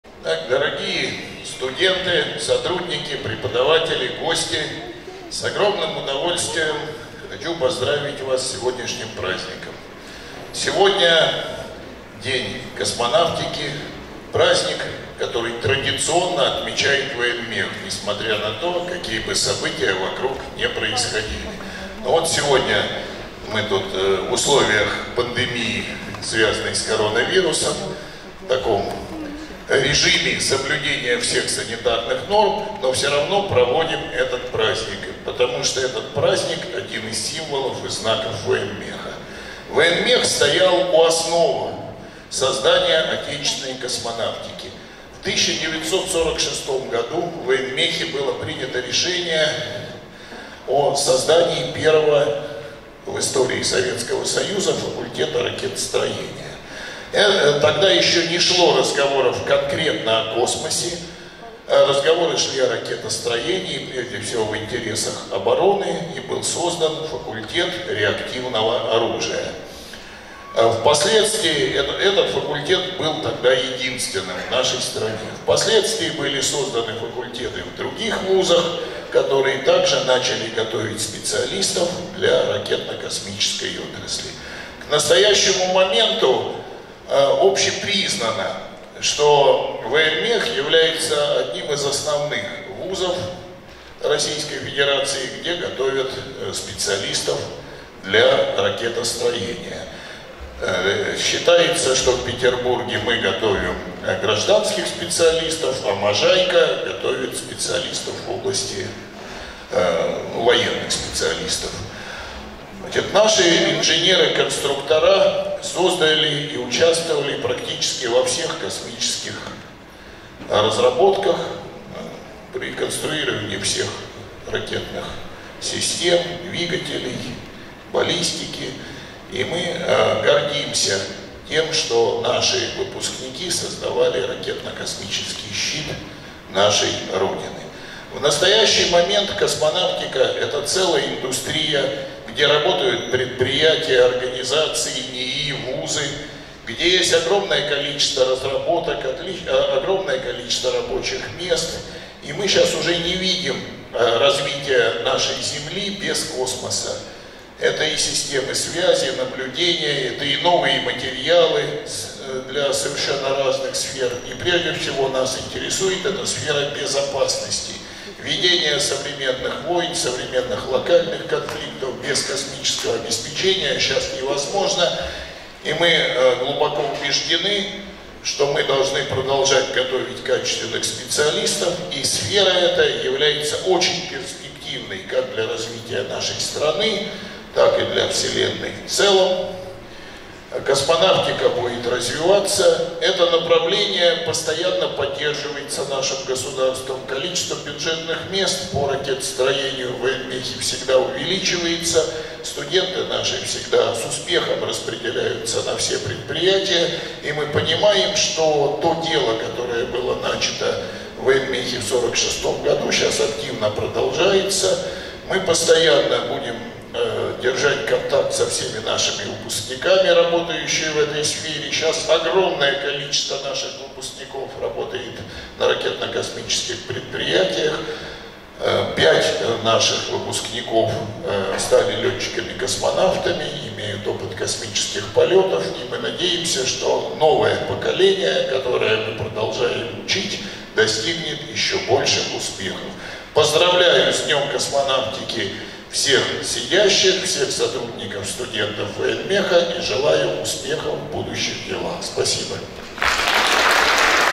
2021-04-12 Ректор БГТУ "ВОЕНМЕХ" им. Д.Ф. Устинова К.М. Иванов 00020
12 апреля 2021 г. праздничный концерт в честь Дня авиации и космонавтики. Актовый зал. БГТУ "ВОЕНМЕХ" им. Д.Ф. Устинова.